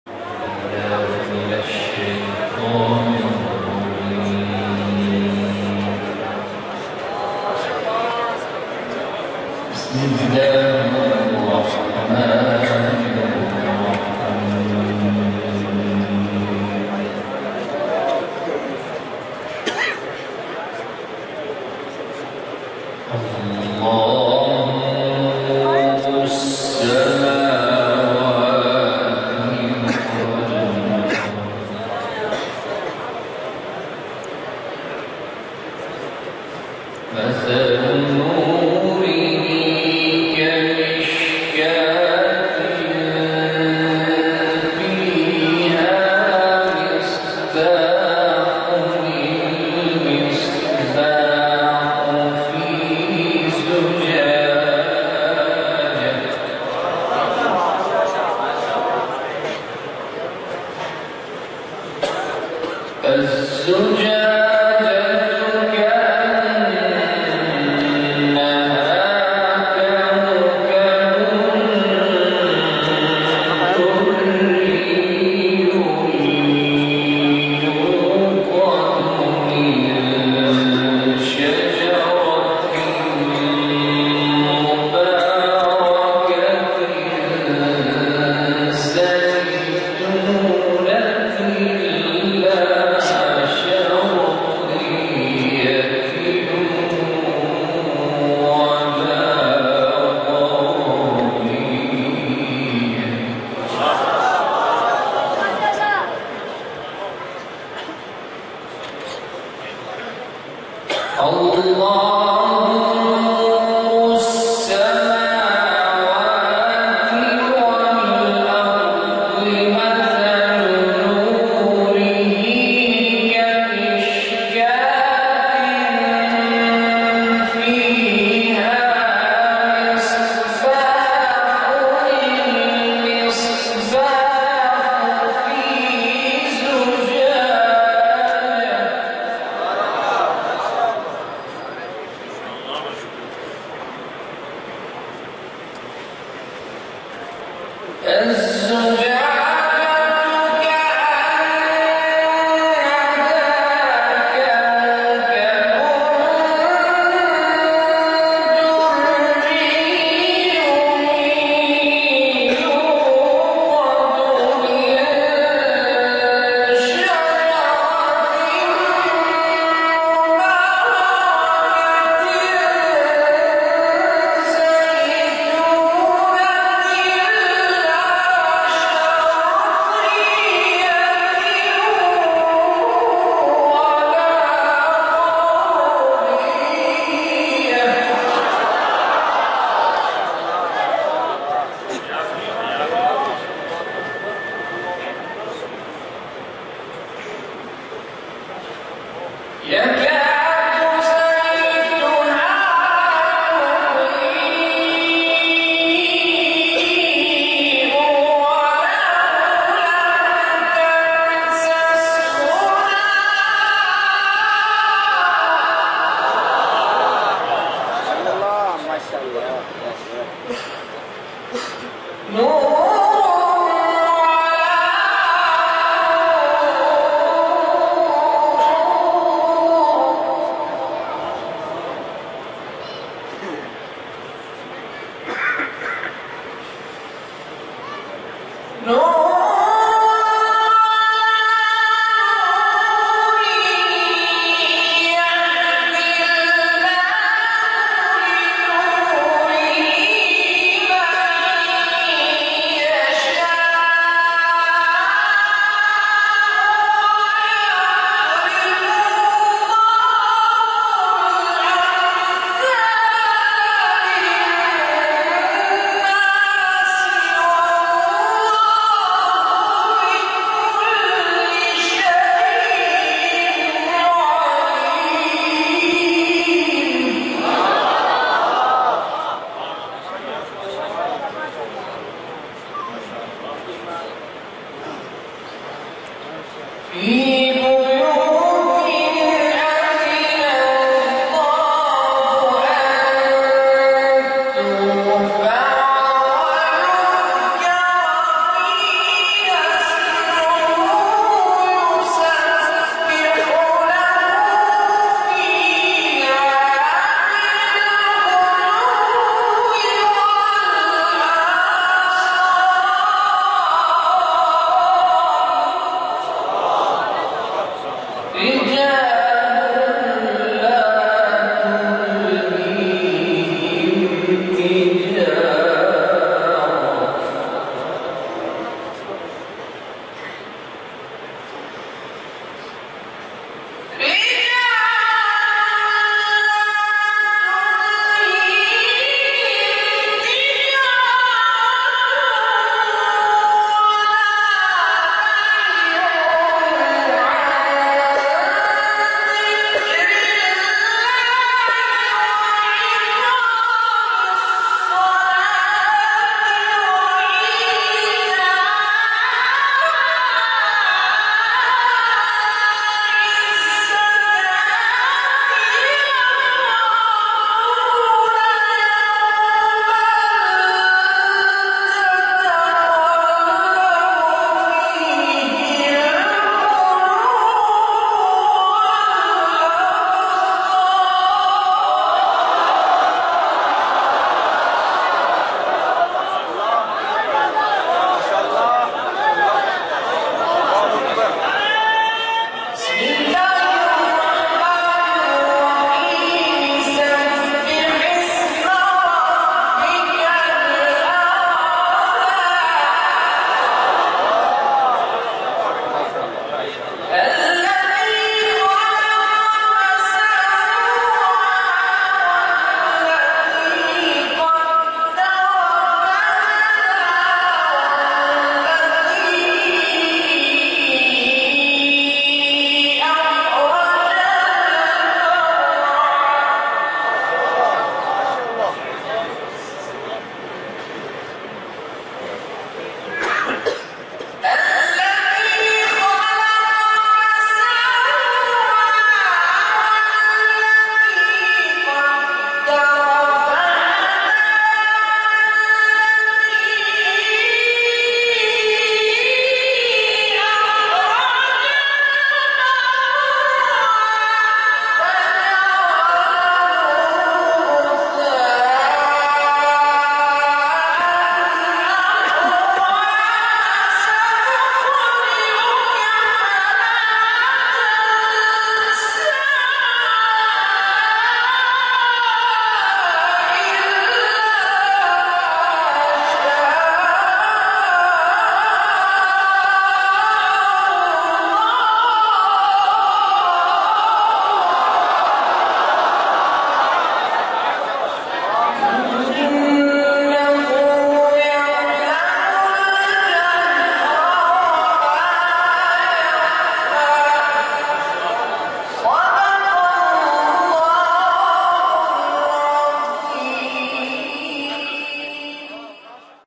Short and Awesome Qari Anwar ul Hassan Shah Bukhari
Rzs Ahmad::::::
Qari Anwar ul Hassan Shah Bukhari Do This Tilwah at Jamia Ashrafia Lahore